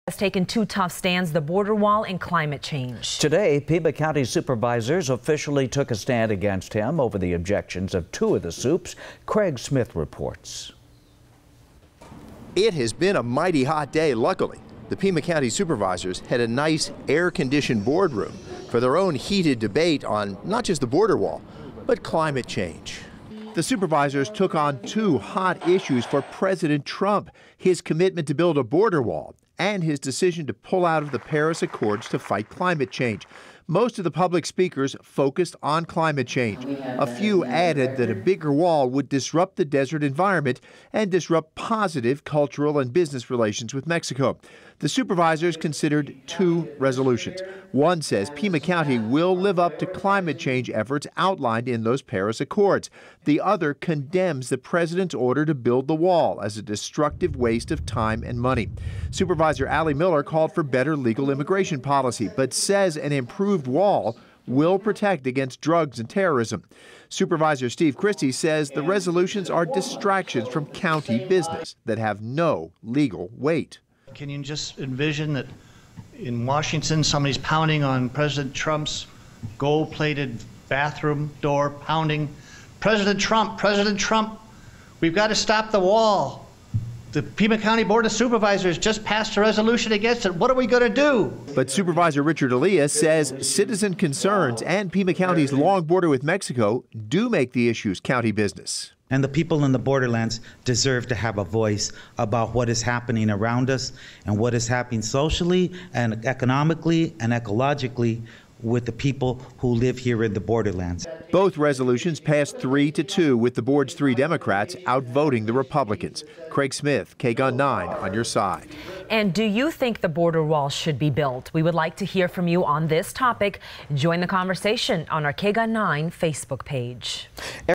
County supes vote to oppose border wall, support Paris climate accords
The Pima County Board of Supervisors passed two resolutions Tuesday geared at opposing White House policies.